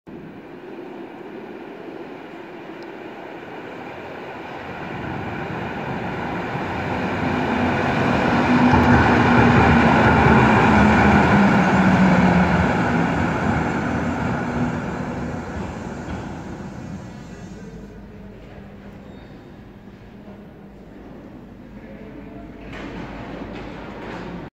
RuhrBahn M-Wagen Linie 108 Einfahrt Rathaus Essen
In diesem Video sehen wir einen M-Wagen der RuhrBahn am Rathaus Essen.

Aufgenommen mit dem Samsung Galaxy S7 Edge.